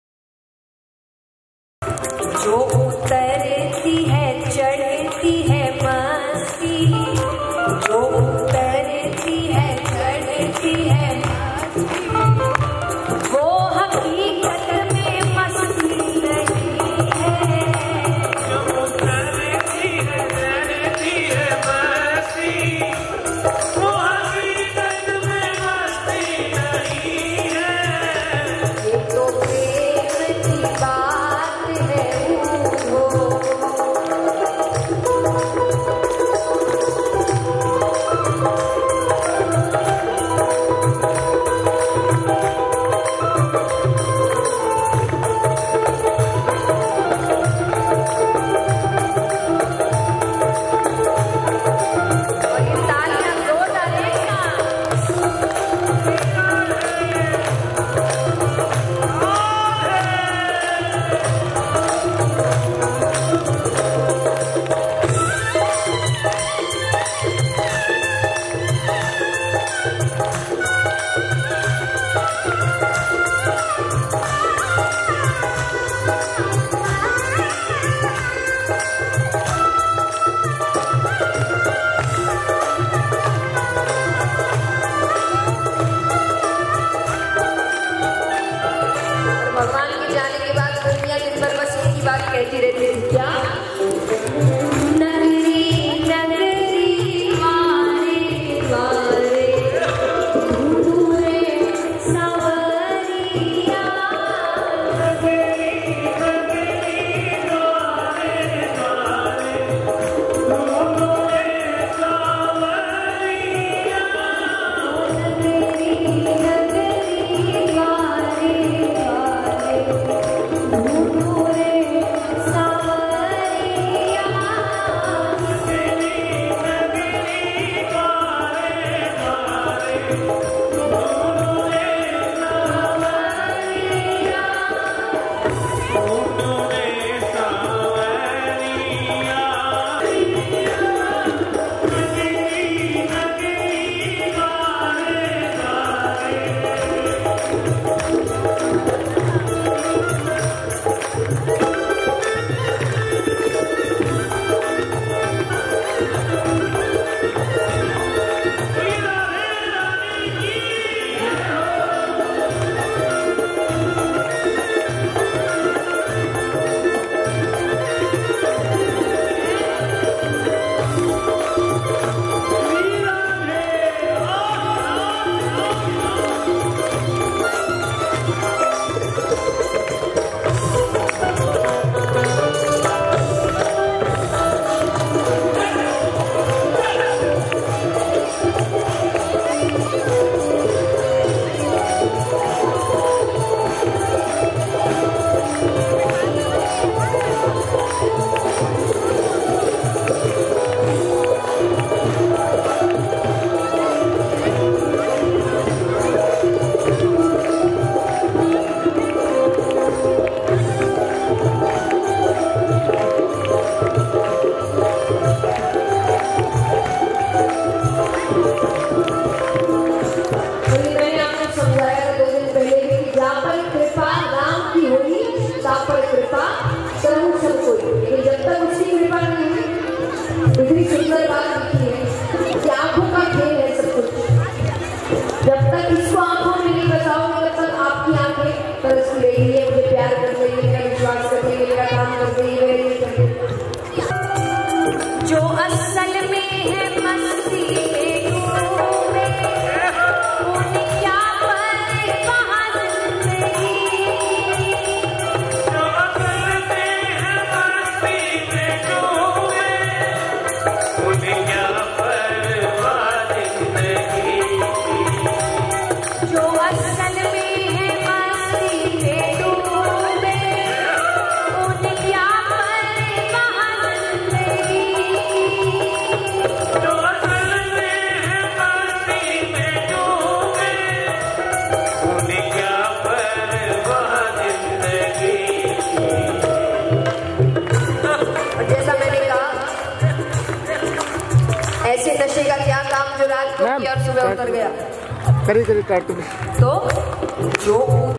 जया किशोरी के भागवत भजनों से झूमा जबलपुर
जबलपुर में चल रहे भागवत का आज 5 वाँ दिन है जहा जया किशोरी के प्रवाचनों को सुनने के लिए दूर दूर से लोग आ रहे है